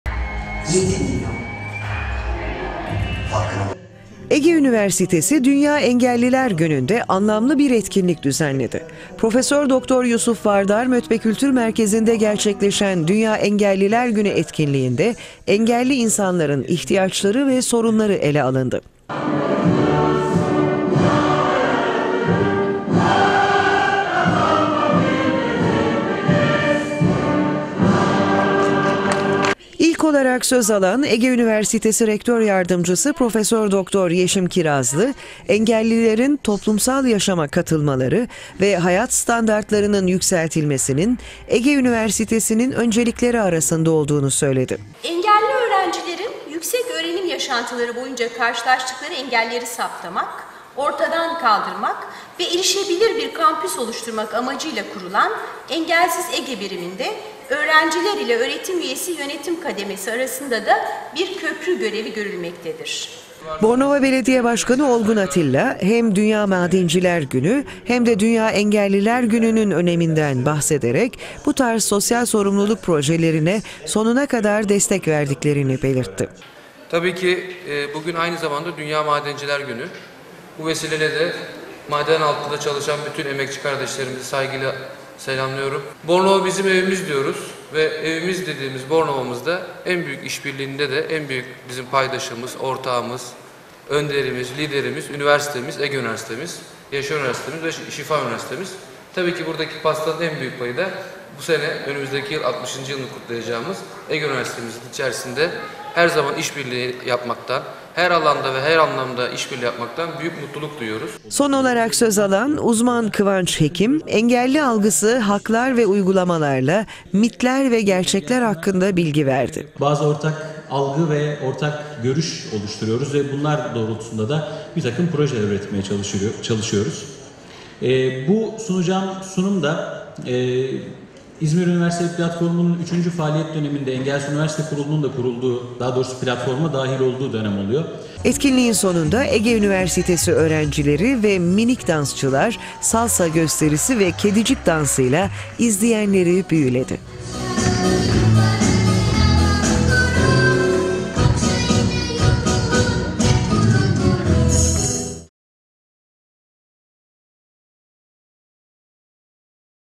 Ege Üniversitesi’nde Engelsiz Günler
Ege Üniversitesi “Dünya Engelliler Günü” kapsamında önemli bir etkinliğe ev sahipliği yaptı. Etkinlikte konuşan Ege Üniversitesi Rektör Yardımcısı Prof. Dr. Yeşim Kirazlı, engellilerin yaşama katılmalarının Ege Üniversitesi’nin öncelikleri arasında yer aldığını belirtti.

Haber: Hasan ÖZYAŞ
Kamera: Hasan Can TARHAN